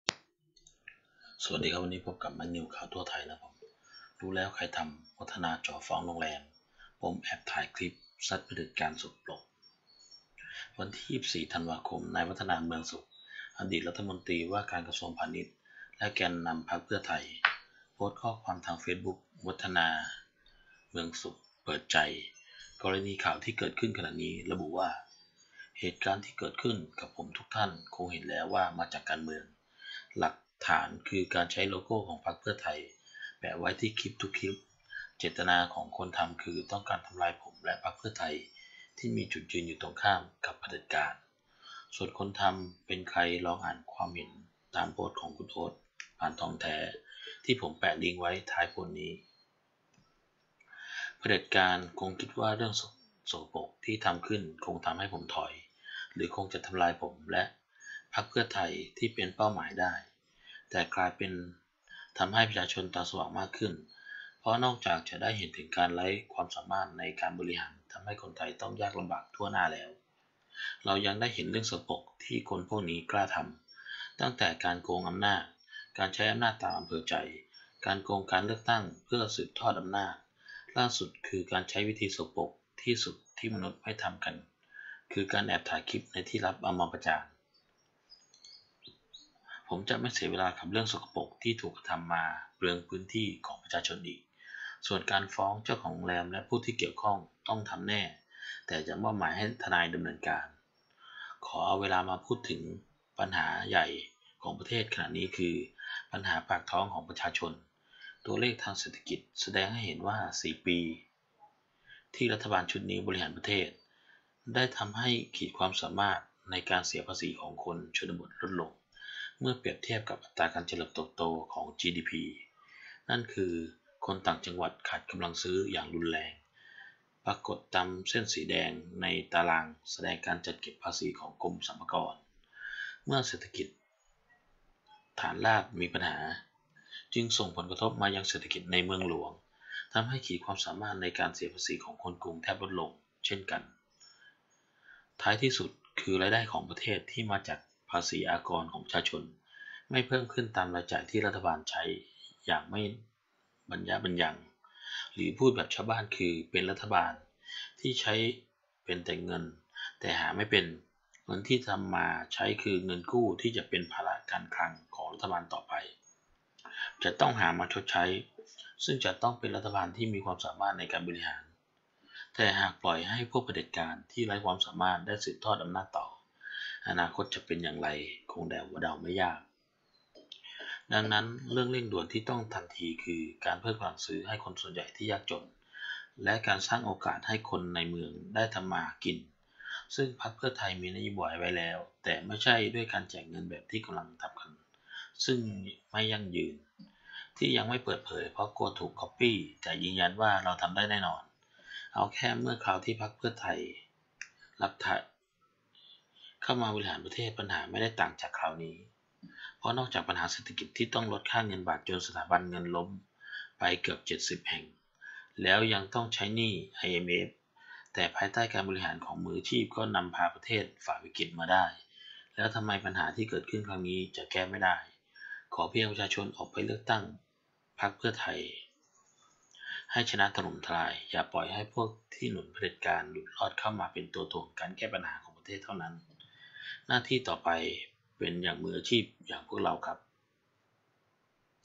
รู้แล้วใครทำ! ‘วัฒนา’ จ่อฟ้องโรงแรม ปมแอบถ่ายคลิป ซัดเผด็จการสกปรก!
รู้แล้วใครทำ! ‘วัฒนา’ – วันนี้ (24 ธ.ค.) นายวัฒนา เมืองสุข อดีตรัฐมนตรีว่าการกระทรวงพาณิชย์ และแกนนำพรรคเพื่อไทย โพสต์ข้อความทางเฟสบุ๊ก Watana Muangsook เปิดใจ กรณีข่าวที่เกิดขึ้นขณะนี้ ระบุว่า

เหตุการณ์ที่เกิดขึ้นกับผมทุกท่านคงเห็นแล้วว่ามาจากการเมือง หลักฐานคือการใช้โลโก้ของพรรคเพื่อไทยแปะไว้ที่คลิปทุกคลิป เจตนาของคนทำคือต้องการทำลายผมและพรรคเพื่อไทยที่มีจุดยืนอยู่ตรงข้ามกับเผด็จการ ส่วนคนทำเป็นใครลองอ่านความเห็นตามโพสต์ของคุณโอ๊ค พานทองแท้ที่ผมแปะลิ้งค์ไว้ท้ายโพสต์นี้

เผด็จการคงคิดว่าเรื่องสกปรกที่ทำขึ้นคงทำให้ผมถอย หรือคงจะทำลายผมและพรรคเพื่อไทยที่เป็นเป้าหมายได้ แต่กลายเป็นทำให้ประชาชนตาสว่างมากขึ้น เพราะนอกจากจะได้เห็นถึงการไร้ความสามารถในการบริหารทำให้คนไทยต้องยากลำบากทั่วหน้าแล้ว

เรายังได้เห็นเรื่องสกปรกที่คนพวกนี้กล้าทำ ตั้งแต่การโกงอำนาจ การใช้อำนาจตามอำเภอใจ การโกงการเลือกตั้งเพื่อสืบทอดอำนาจ ล่าสุดคือการใช้วิธีสกปรกที่สุดที่มนุษย์ไม่ทำกันคือการแอบถ่ายคลิปในที่ลับเอามาประจาน

ผมจะไม่เสียเวลากับเรื่องสกปรกที่ถูกกระทำมาเปลืองพื้นที่ของประชาชนอีก ส่วนการฟ้องเจ้าของโรงแรมและผู้ที่เกี่ยวข้องต้องทำแน่แต่จะมอบหมายให้ทนายดำเนินการ

ขอเอาเวลามาพูดถึงปัญหาใหญ่ของประเทศขณะนี้คือปัญหาปากท้องของประชาชน ตัวเลขทางเศรษฐกิจแสดงให้เห็นว่า 4 ปีเศษที่รัฐบาลชุดนี้บริหารประเทศ ได้ทำให้ขีดความสามารถในการเสียภาษีของคนชนบทลดลงเมื่อเปรียบเทียบกับอัตราการเจริญเติบโตของ GDP นั่นคือคนต่างจังหวัดขาดกำลังซื้ออย่างรุนแรง ปรากฏตามเส้นสีแดงในตารางแสดงการจัดเก็บภาษีของกรมสรรพากร